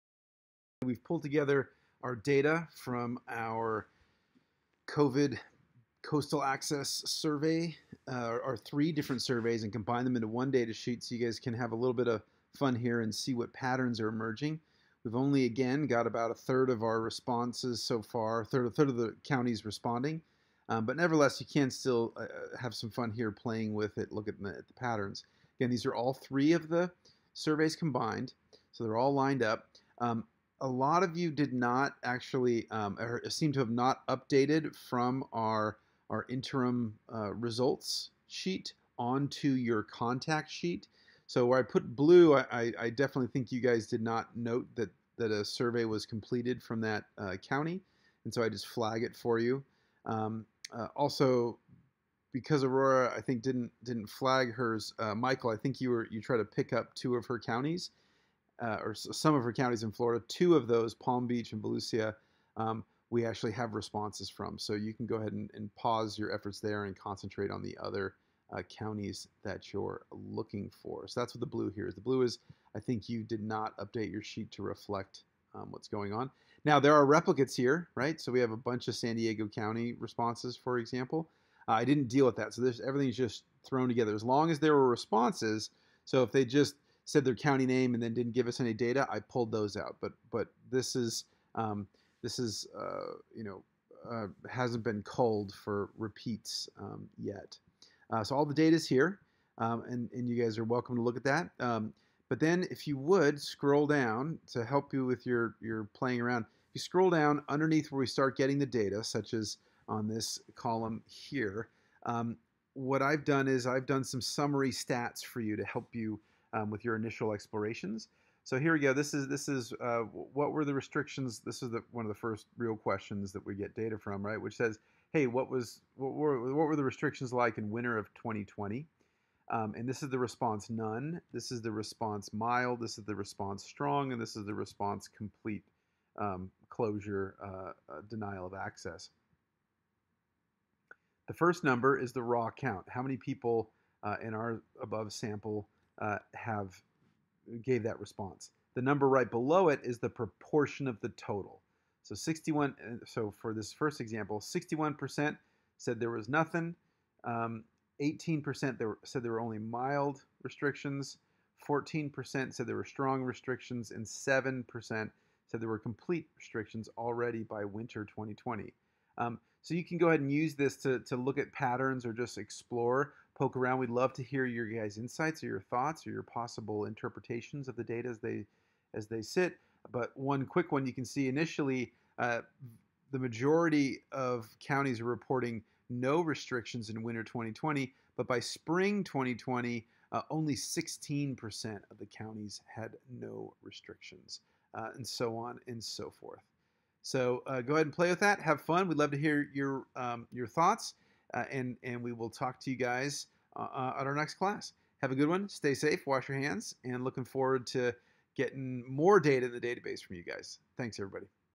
Beachless Coastal Access Interim Data
This was our raw data as on May 12, 2021, with duplicates, etc. not yet removed. This represents data from approximately 1/3 of all U.S. counties during the COVID-19 Pandemic.